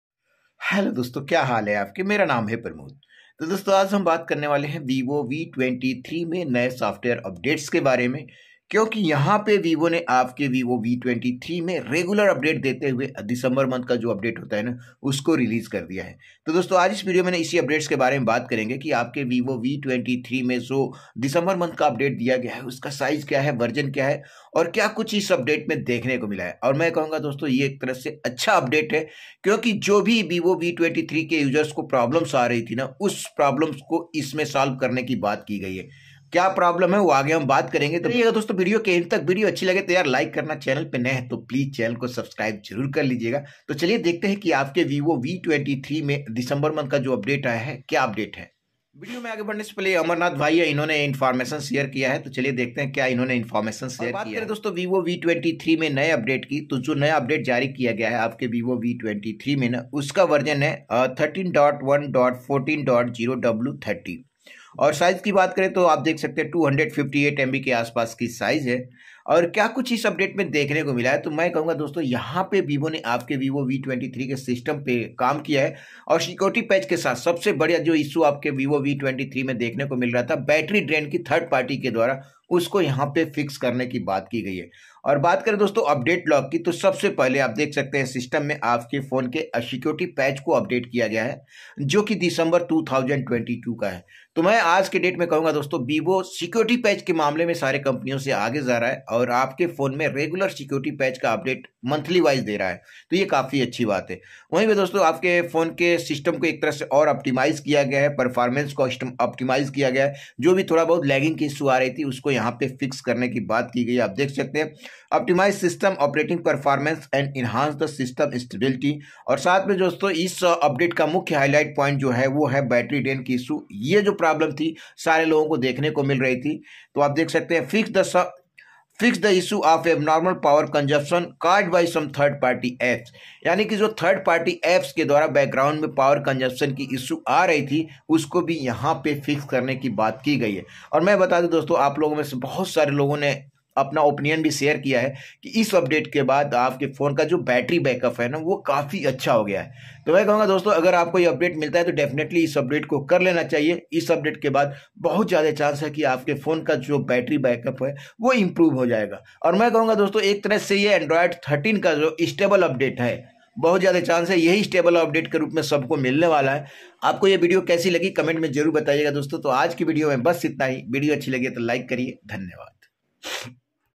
Vivo V23 New Update December 2022 | Vivo V23 Android 13 Stable Update Released
Namsakar dosto Bhaiya G Tech me app sabhi ka sawagat hai ,Aj is Video me Bataya hu, new software update in Vivo V23 December 2022 this is the stable update of Android 13 for Vivo V23 uses this is good update because in this update battery backup improved by many users
I hope you enjoy this video and don't forgot like 👍👍this video,

Equipments use by me for video👇

👉Tripod for both camara and Mobile